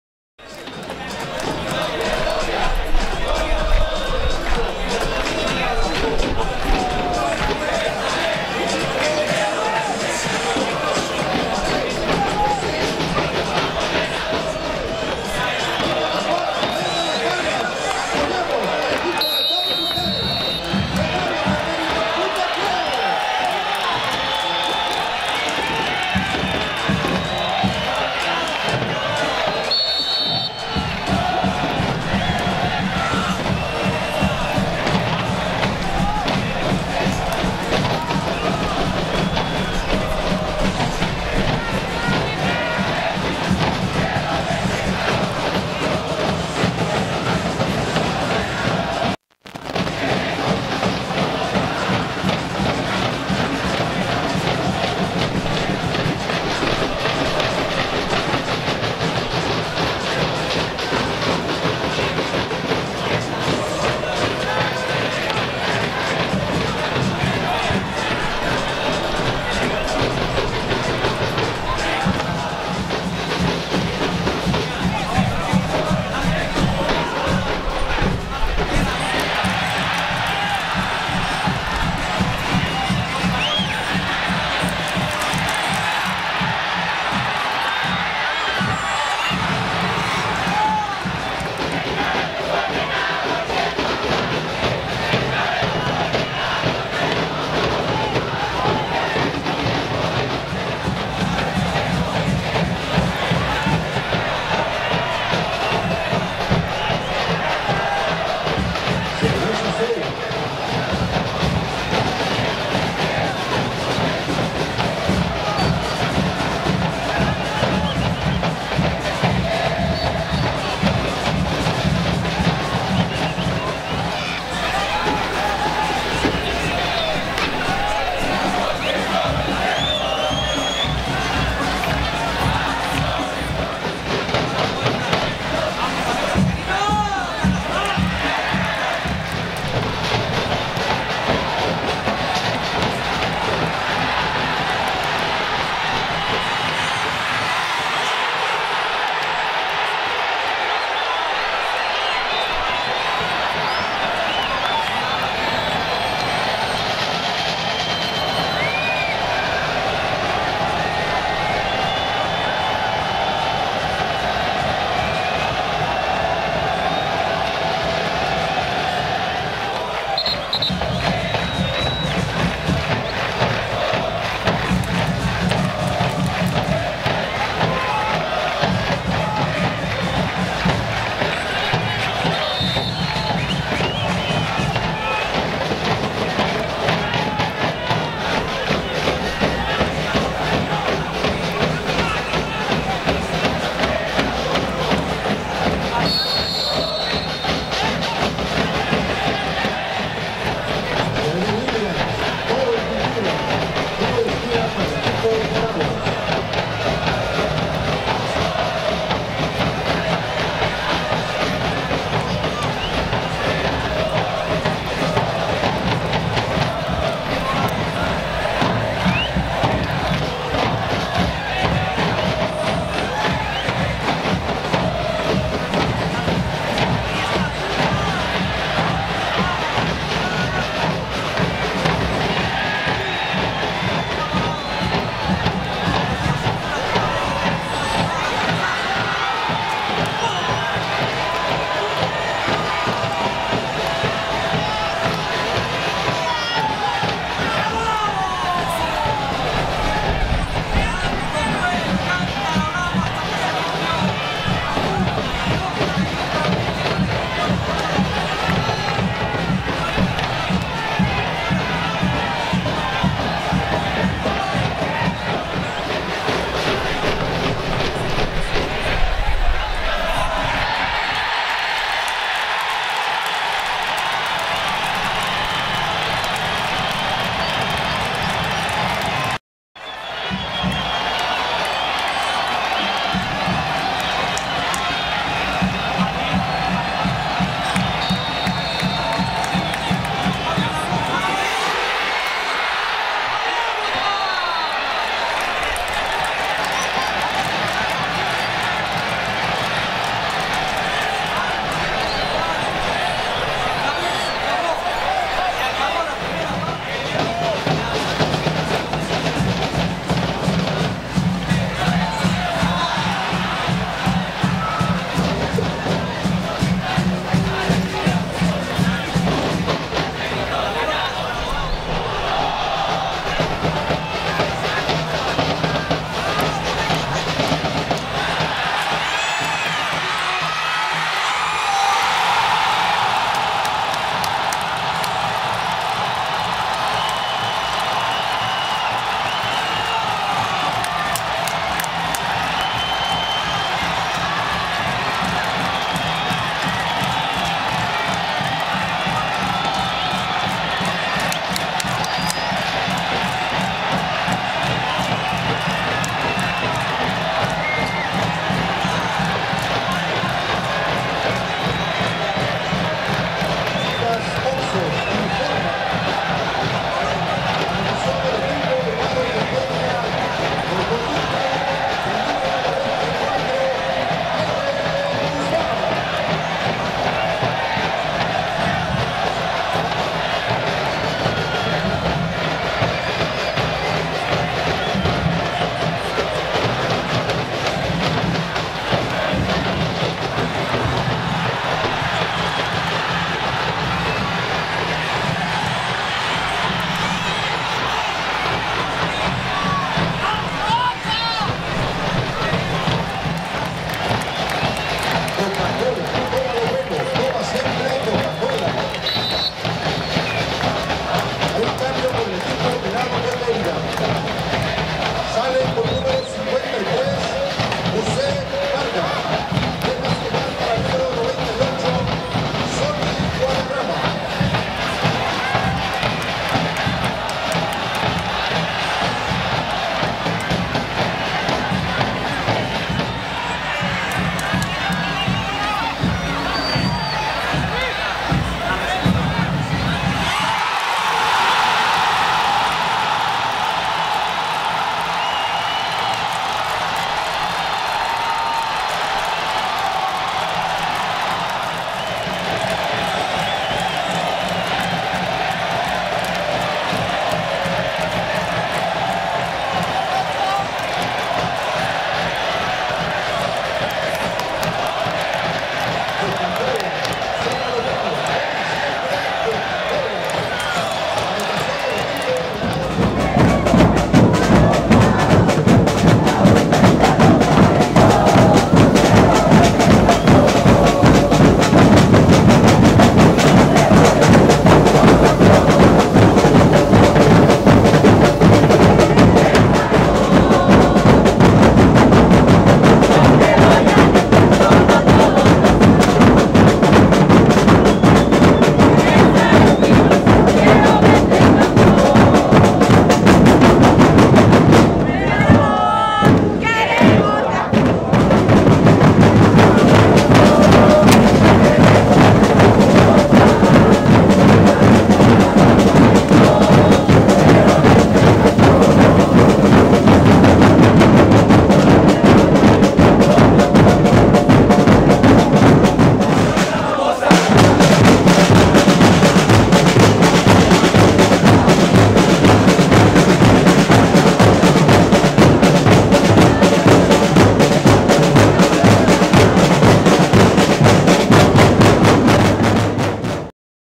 Venados de Merida vs Dorados J10 Ap2009 LDA
Algunas imagenes del encuentro entre Merida FC y Dorados fecha 10 de la Liga de Ascenso en el Carlos Iturralde.